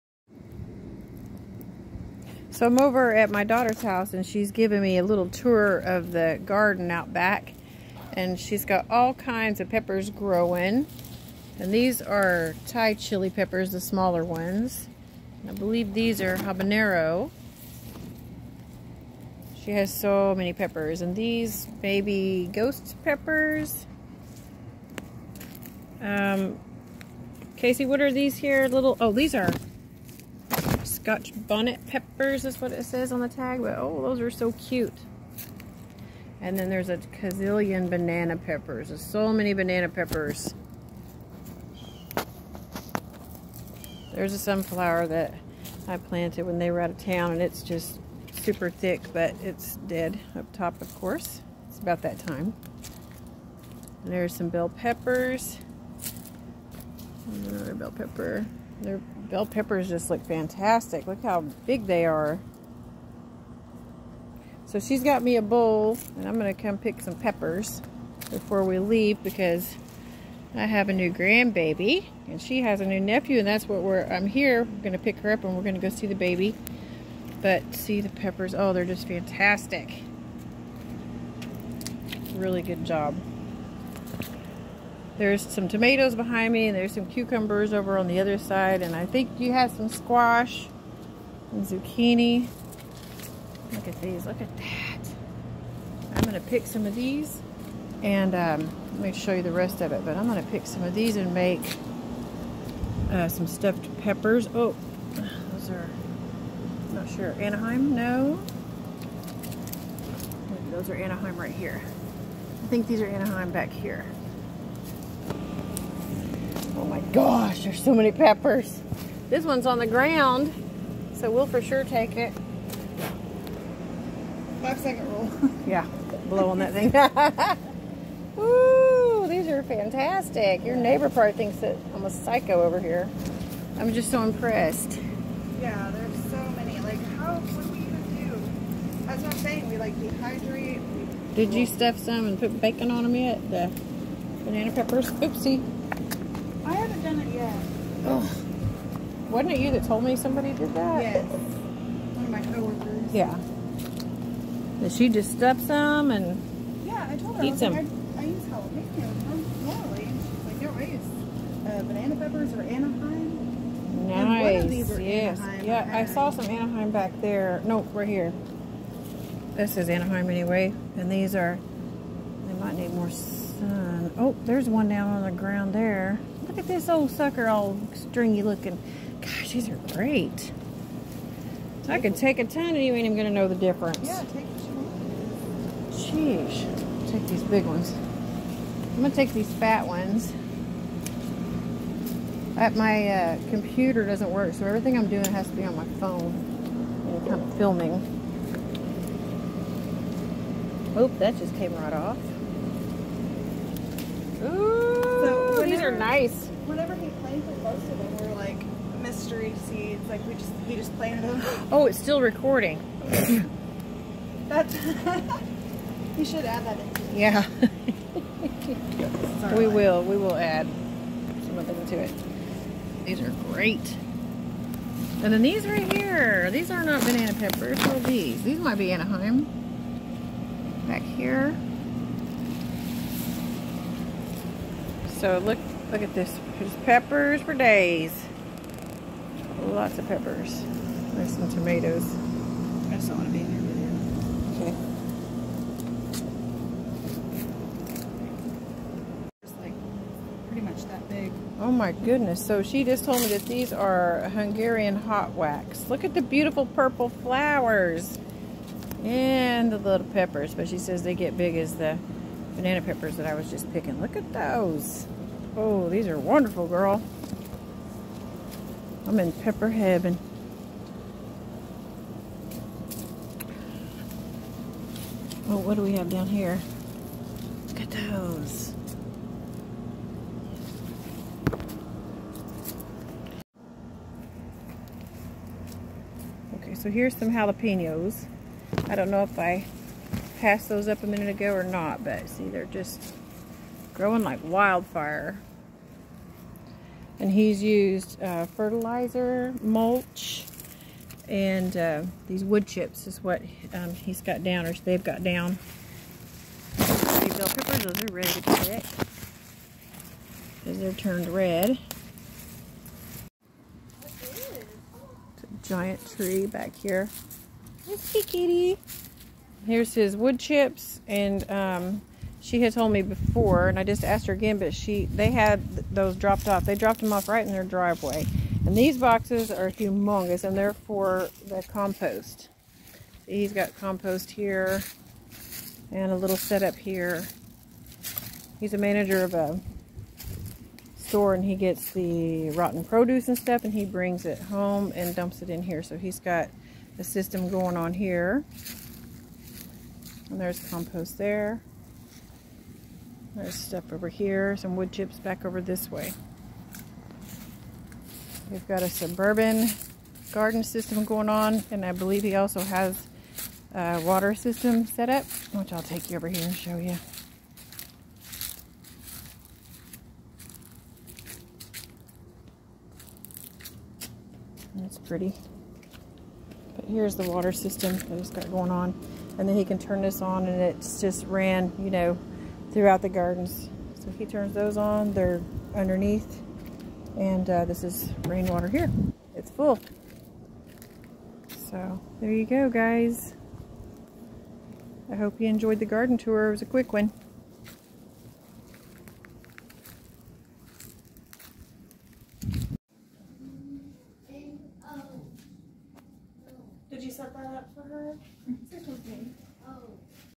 A Garden Tour of My Daughter & Son In Law's Pepper Garden. 🌶updated version
Tour of my daughter and son in laws pepper garden. 🌶